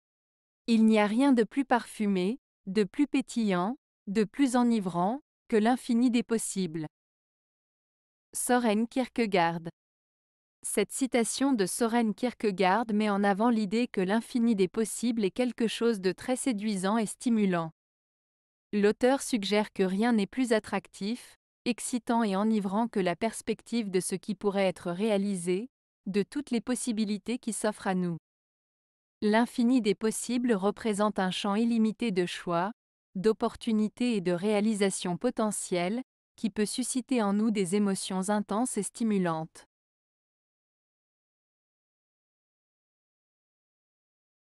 Søren Kierkegaard - Il n'y a rien de plus parfumé, de plus pétillant, de plus enivrant, que l'infini
Cette citation de Søren Kierkegaard met en avant l'idée que l'infini des possibles est quelque chose de très séduisant et stimulant. L'auteur suggère que rien n'est plus attractif, excitant et enivrant que la perspective de ce qui pourrait être réalisé, de toutes les possibilités qui s'offrent à nous. L'infini des possibles représente un champ illimité de choix, d'opportunités et de réalisations potentielles, qui peut susciter en nous des émotions intenses et stimulantes.